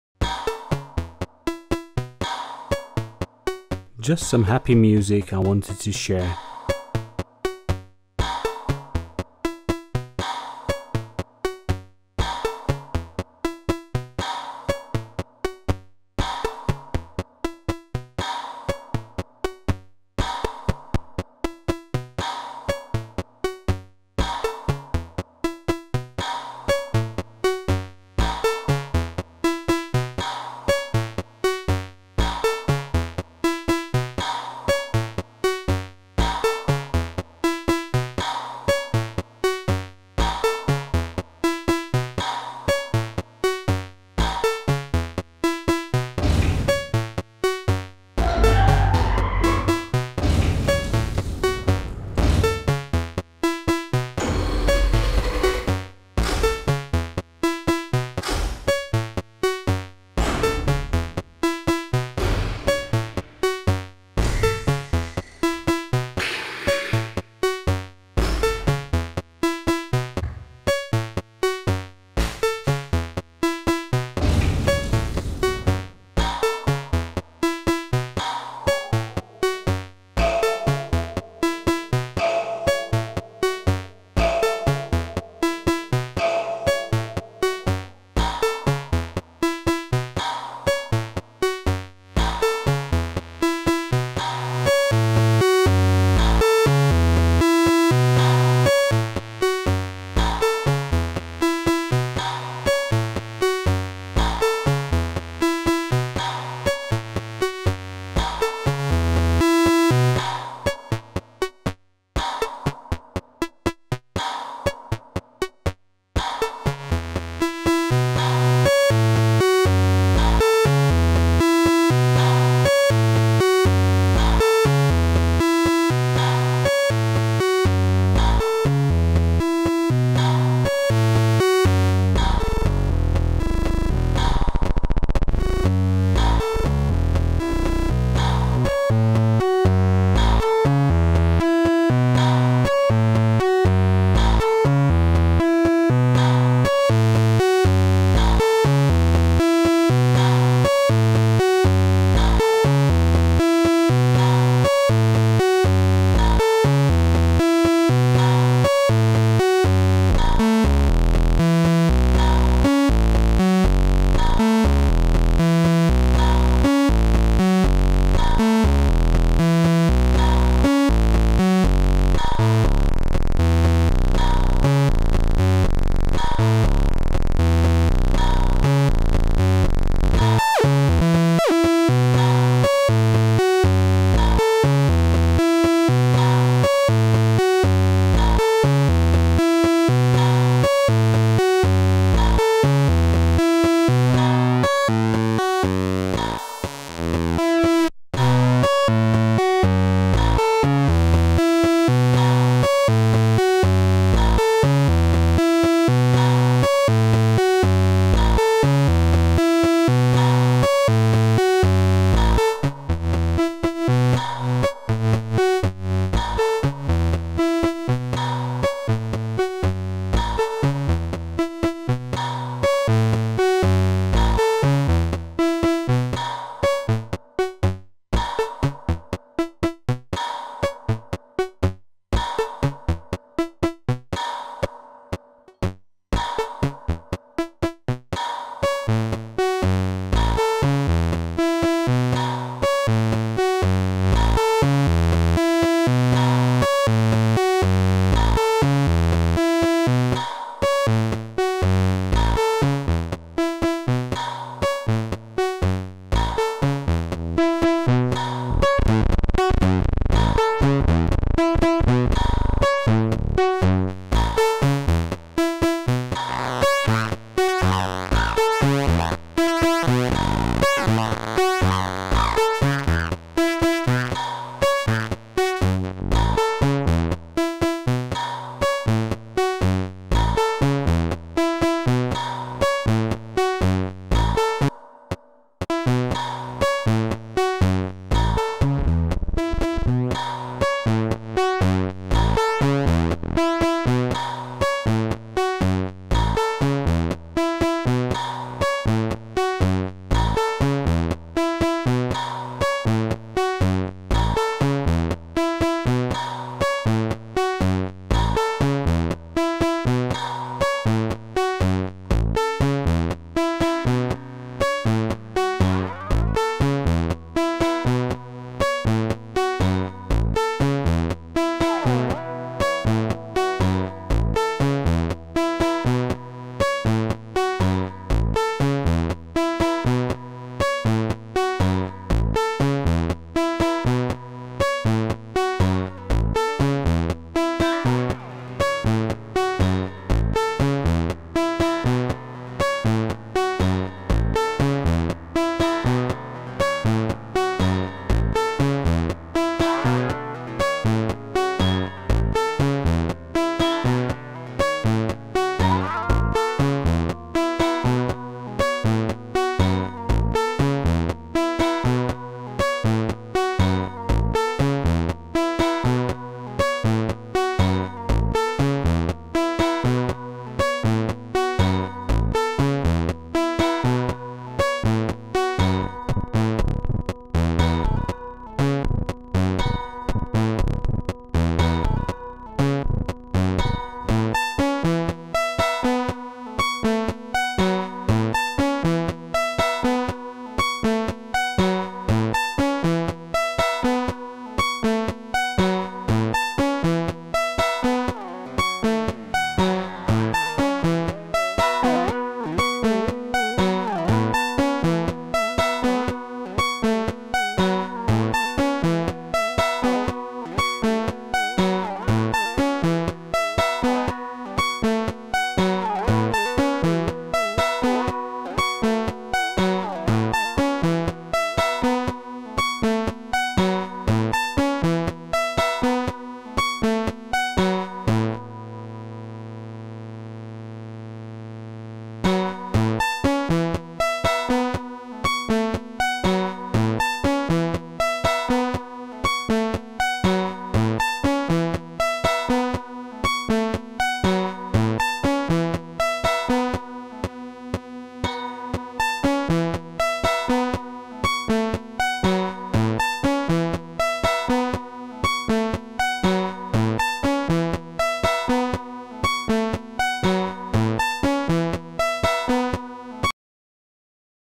Monday Night Jam | Eurorack Modular Electropunk
Just a nice jam session. Hope you like it.